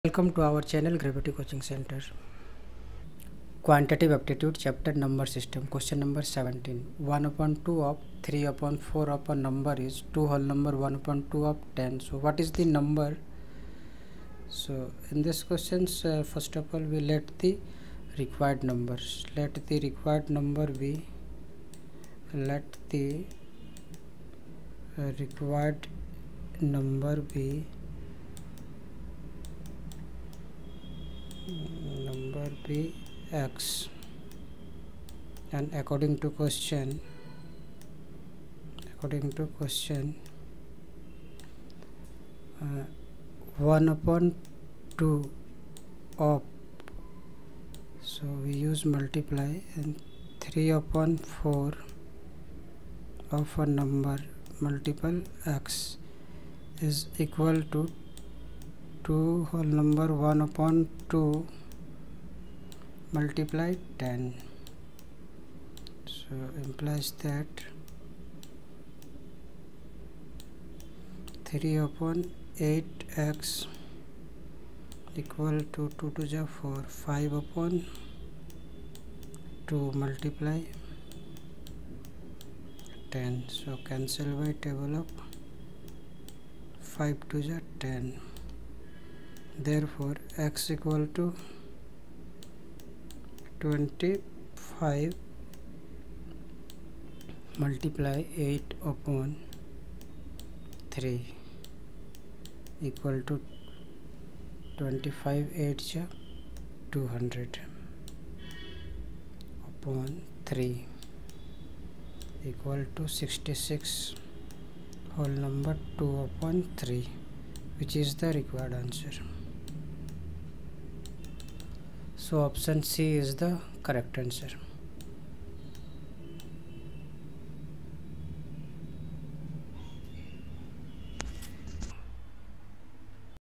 Q17 | 1/2 of 3/4 of a number is 2(1/2) of 10. What is the number? | Number System
Qu(17):- 1/2 of 3/4 of a number is 2(1/2) of 10. What is the number?